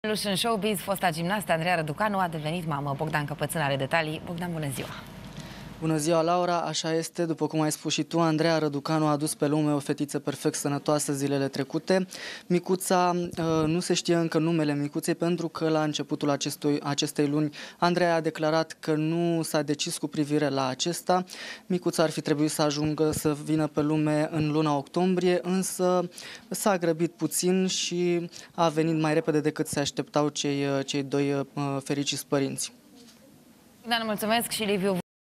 Andreea Răducanu a devenit mamă!
Un nou bebeluş în showbiz! Fosta gimnastă Andreea Răducanu a devenit mamă!